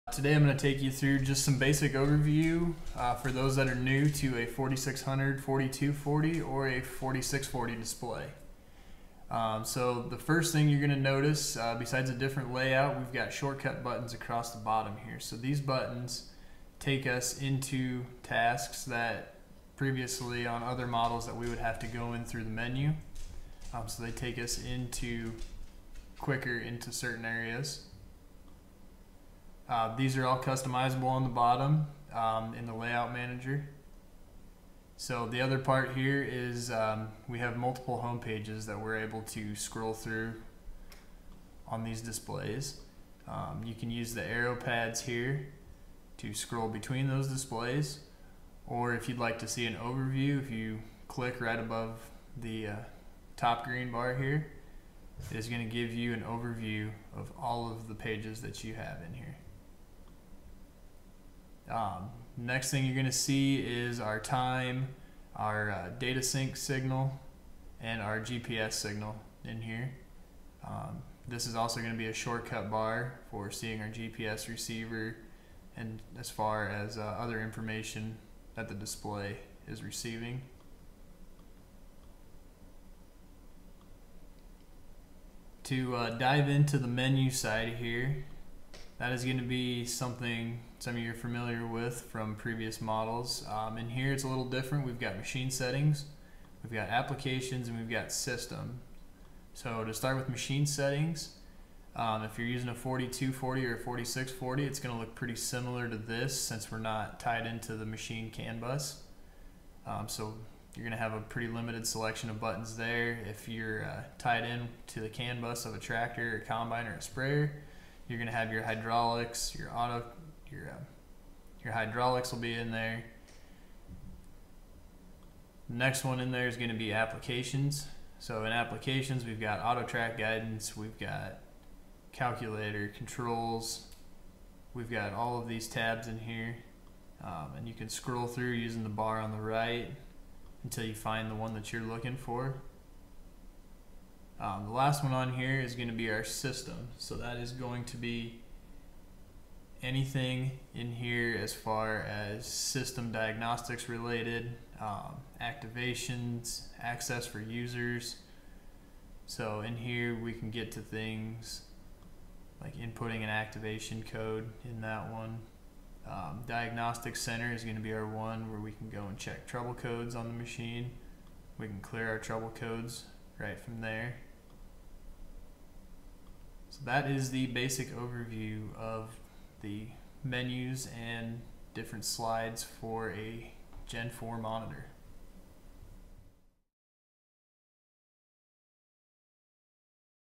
Basic Overview and Operation of John Deere Gen 4 Display for Planting
New to the John Deere Gen 4 display? Here are the basics you need to know before you go to the field. Need more help? Call your Platte Valley Equipment precision ag specialist in Clarkson, Fremont, Humphrey and Wahoo!